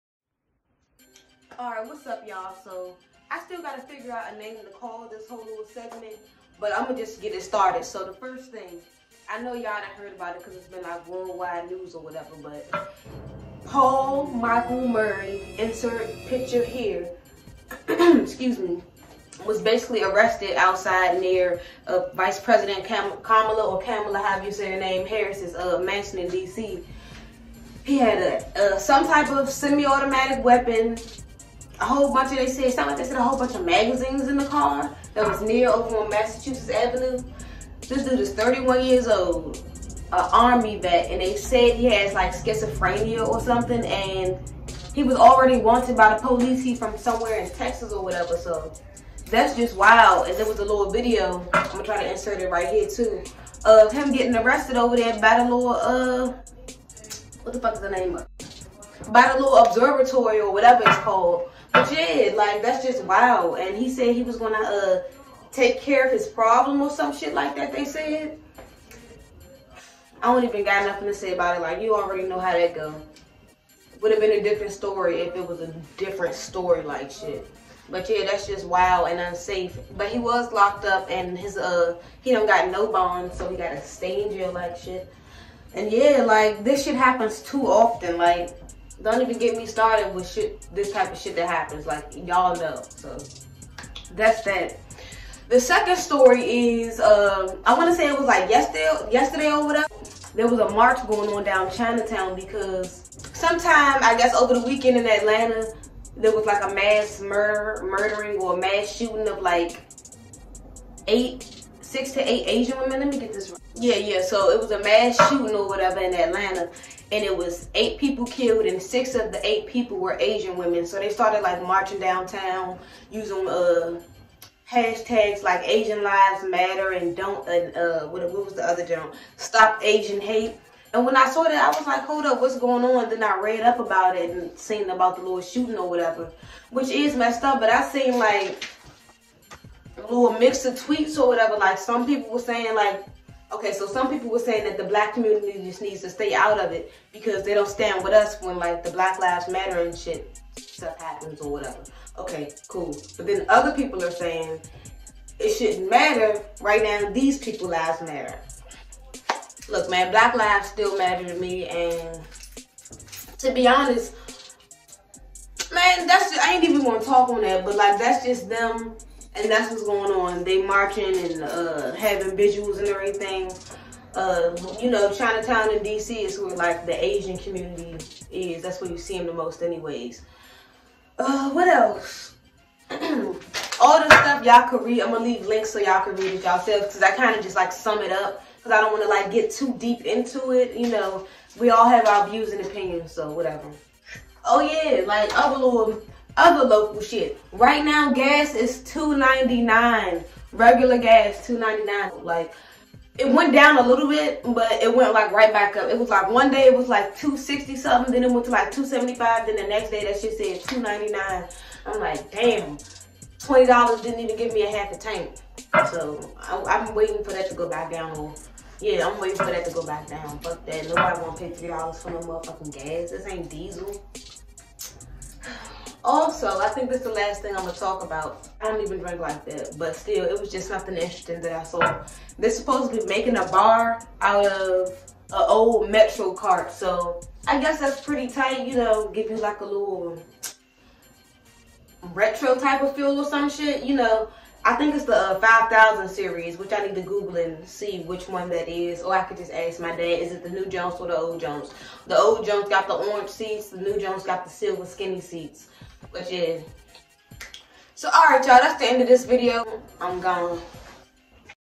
Texas Man Arrested Near VP Kamala Harris House, stopasianhate protest & Gas Prices Up| 202News
Wild week for DC, Texas Man arrested outside of VPs house in DC , Asian protest in Chinatown stopasianhate. Gas prices oc for no reason.